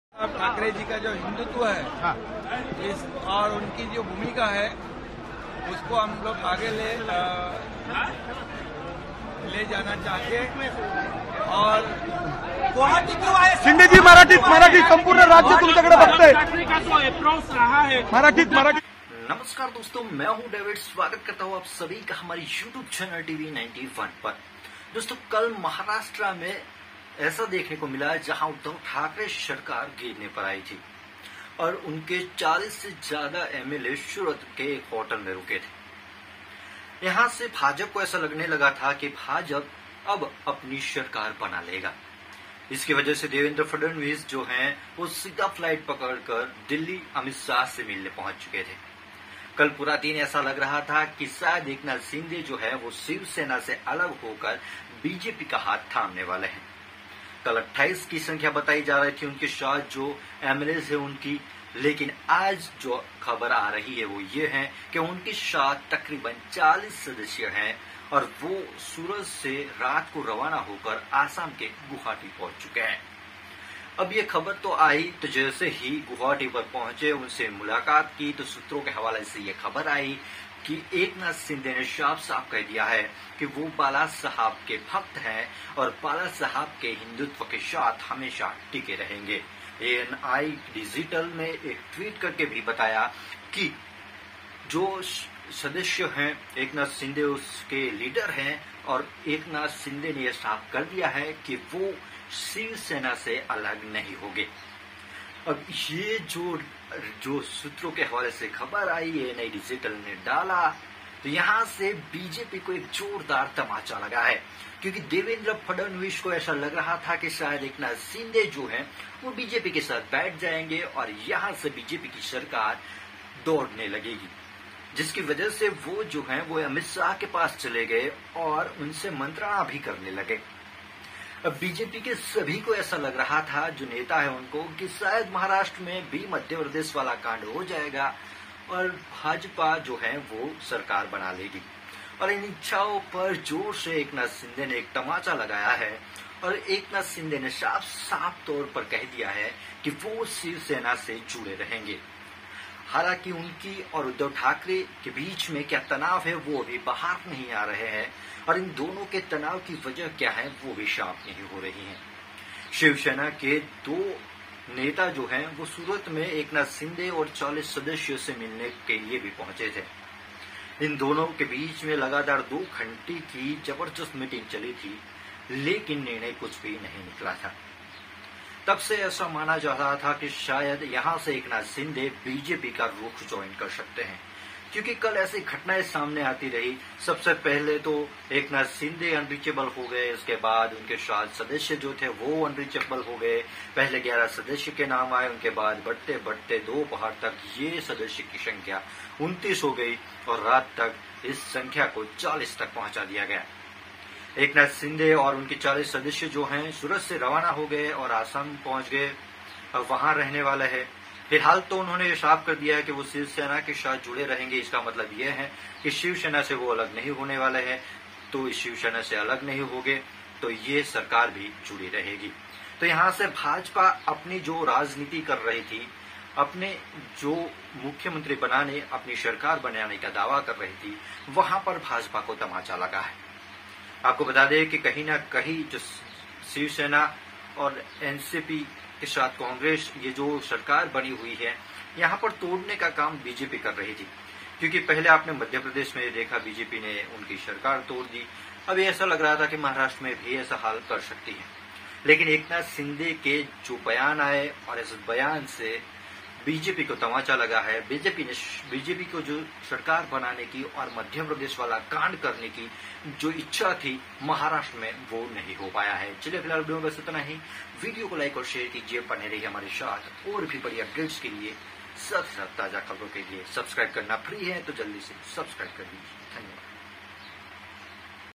Maharashtra Political Crisis : Eknath Shinde का BJP को जोरदार तमाचा, नही छोड़ेंगे ShivSena । TV91
Eknath Shinde का BJP को जोरदार तमाचा, नही छोड़ेंगे ShivSena । Uddhav Thakrey । TV91

TV91 covers news from politics , entertainment, sports, bussiness etc.